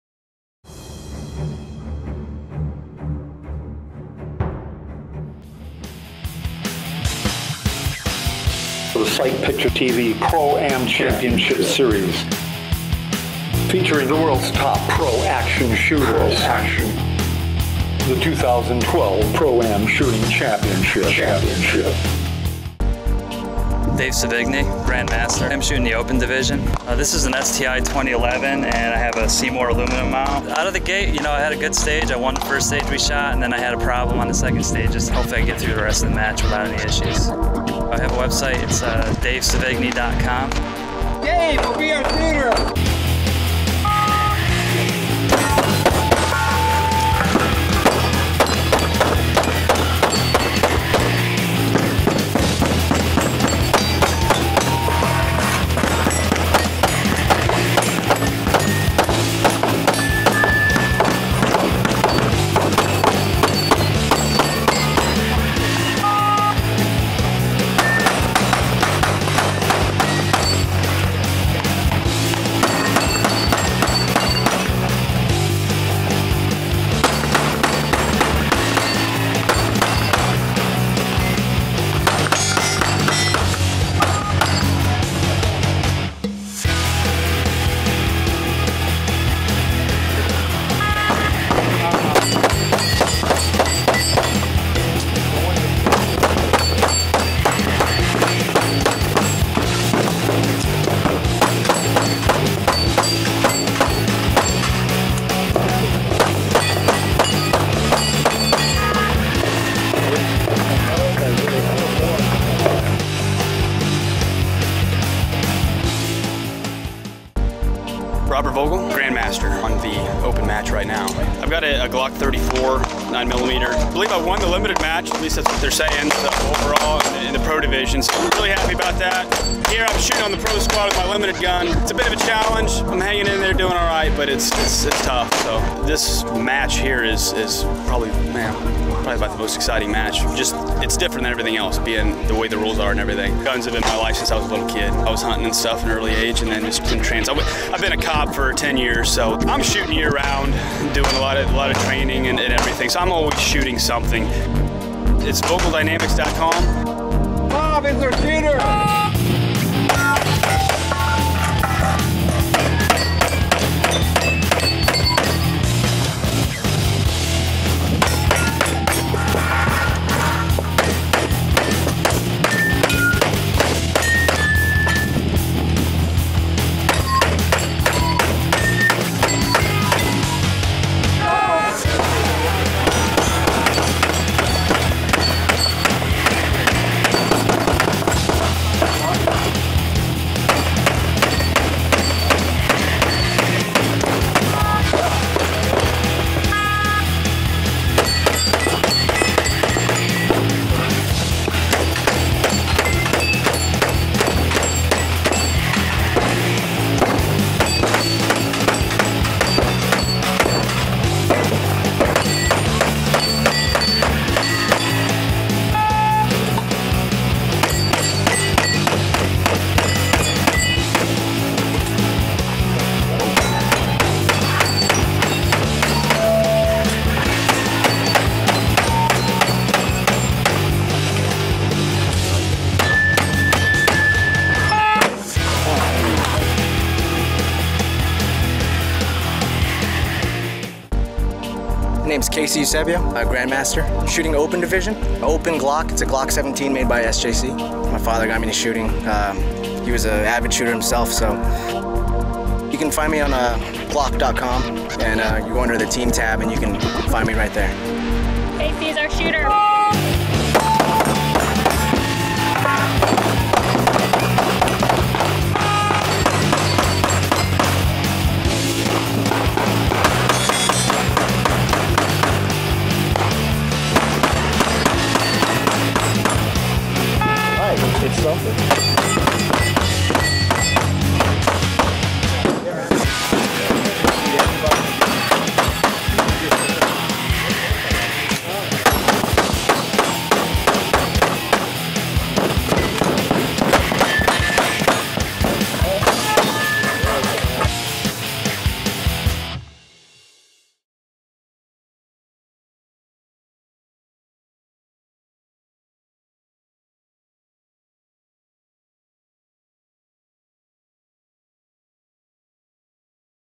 Pro-Am Shooting 2012 Championship Series Episode 1
Site Picture TV presents coverage of Pro-Am 2012 Shooting Championship 4-video series. See the World's Top Shooters in action -- Max Michel, Blake Miguez, Bob Vogel, KC Eusebio, Frank Garcia and more! Episode 1 features Bob Vogel, Dave Sevigny and KC Eusebio. Three full days of PRO-ACTION SHOOTING with 300 targets, 9 stages. See it all! Don't miss the best pro-action shooting available to view only on Site Picture TV! Produced by Ron Howland